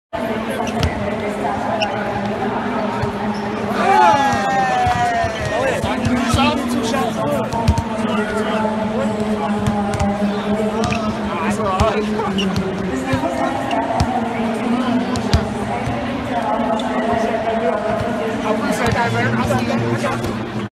Nipsey Hussle @ Lakers Game
Nipsey Hussle hitting threes at Lakers game